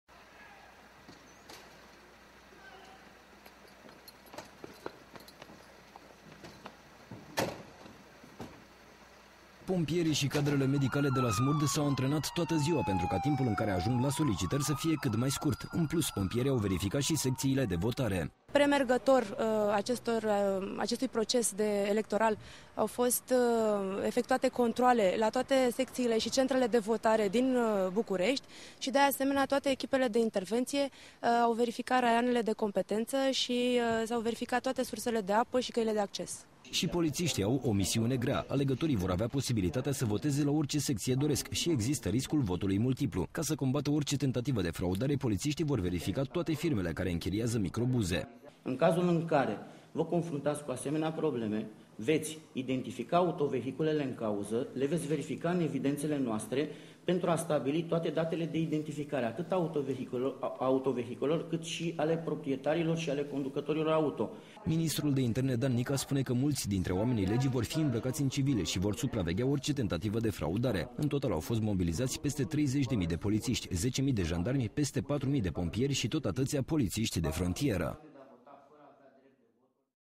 Autorităţile s-au mobilizat pentru alegerile europarlamentar
Autorităţile s-au mobilizat pentru alegerile europarlamentare
50.000 de angajaţi ai Ministerului de Interne, poliţişti, pompieri, jandarmi şi medici vor fi la muncă